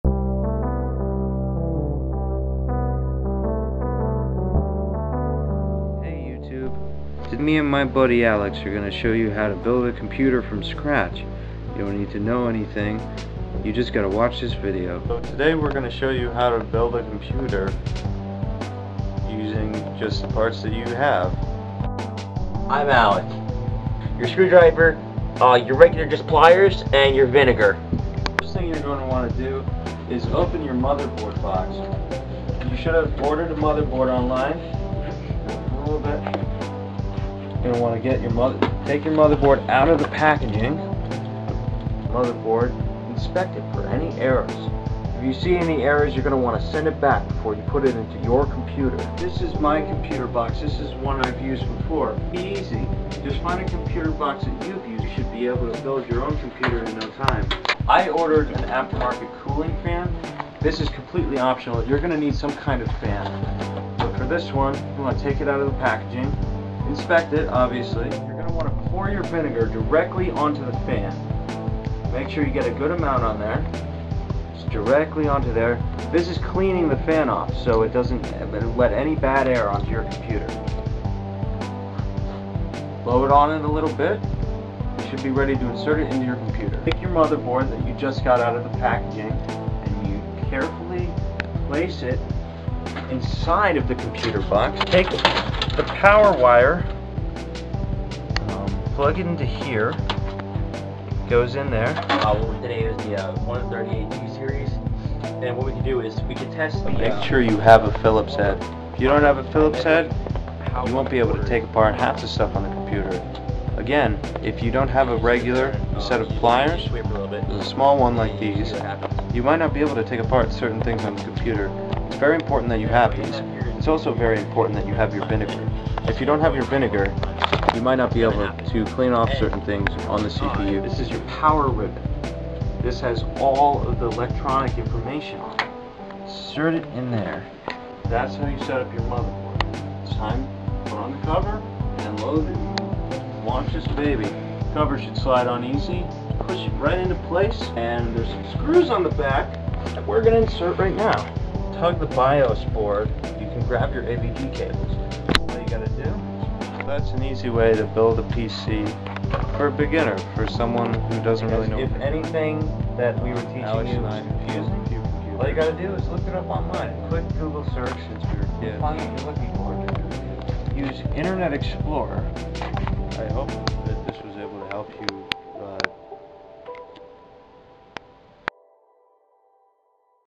how to build a pc easy
new to your computer? want to build one if your own? check out this video for a simple way to build a new computer from basic parts